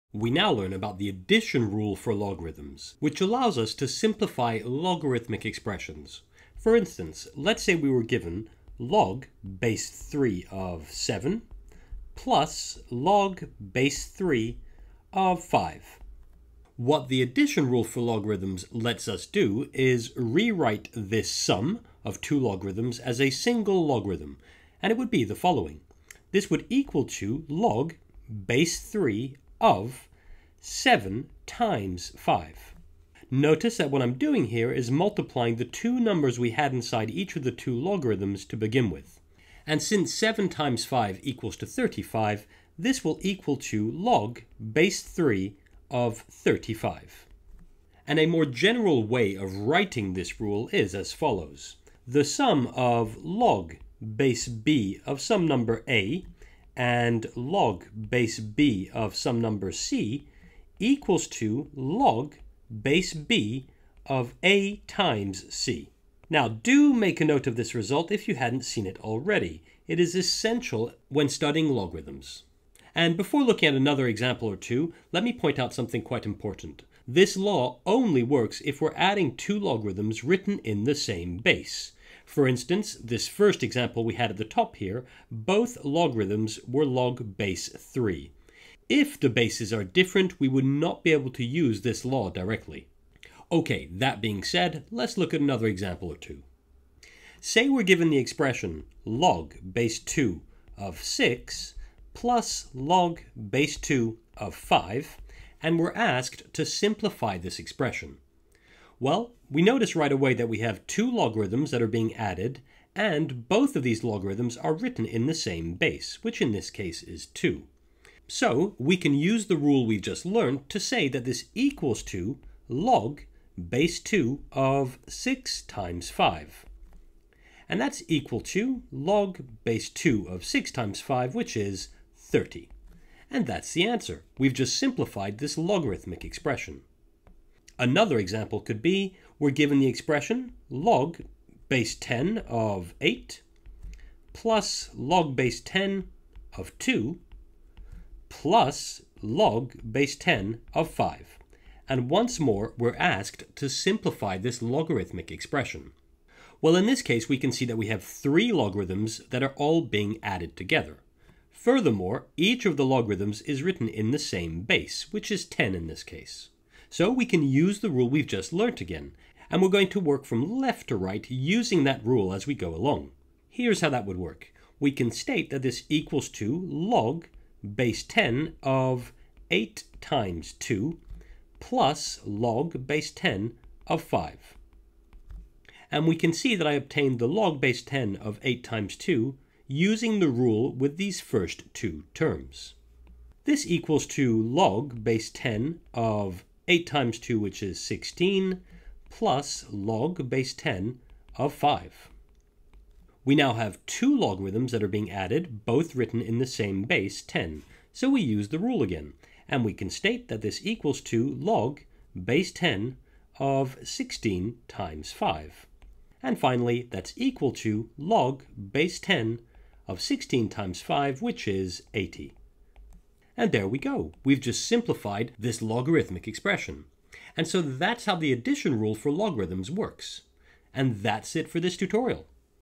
Laws of Logarithms - Addition Rule - Simplifying Logarithmic Expressions - Tutorial 6
The first law of logarithms we learn is the addition rule. This allows to simplify the sum of two logarithms, written in the same base. We end up with one single logarithm: the logarithm of the product of the two initial input values. In other words when we add the log of a number a, with the log of a number b it's equal to the log of a times b.

*** LINKS ***

Lesson Notes, Exercises and Tutorials at RadfordMathematics.com